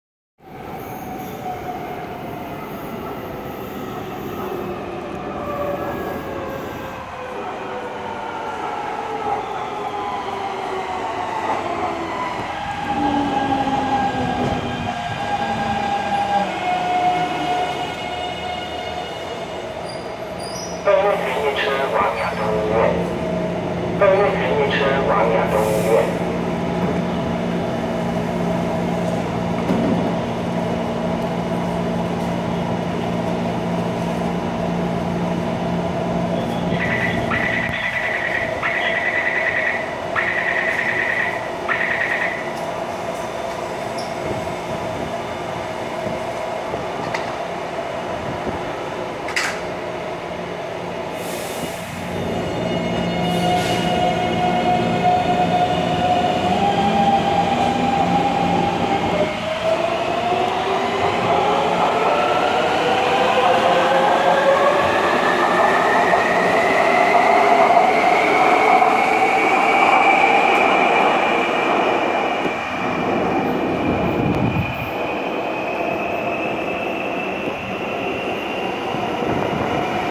台北捷運彩繪列車C321型101/102車次206[朱羅紀世界]永春進出站
5號板南線
往亞東醫院